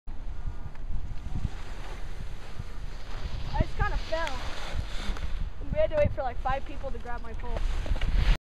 Dennis on the Slopes
Dennis making some nice turns at Mt. High.